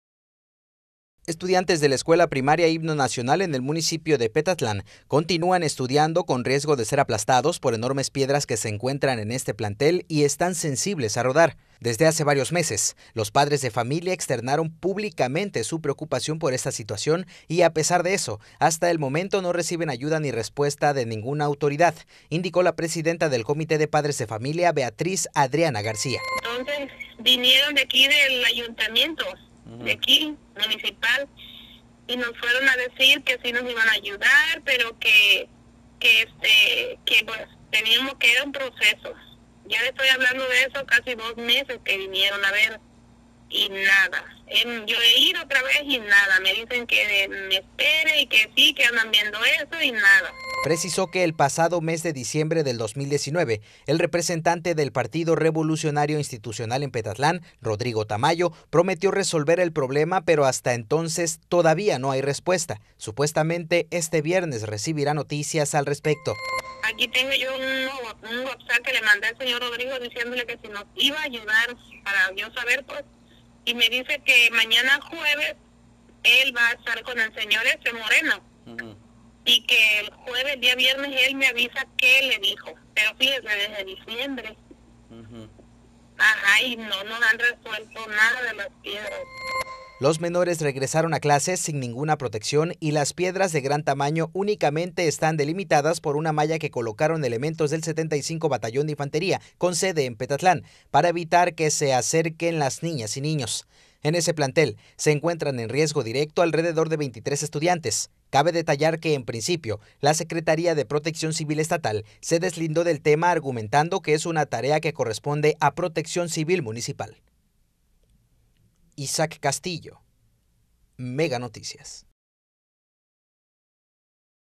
Estudiantes en Petatlán continúan en riesgo por enormes piedras
Estudiantes de la escuela primaria Himno Nacional en el municipio de Petatlán continúan estudiando con riesgo de ser aplastados por enormes piedras que se encuentran en este plantel y están sensibles a rodar.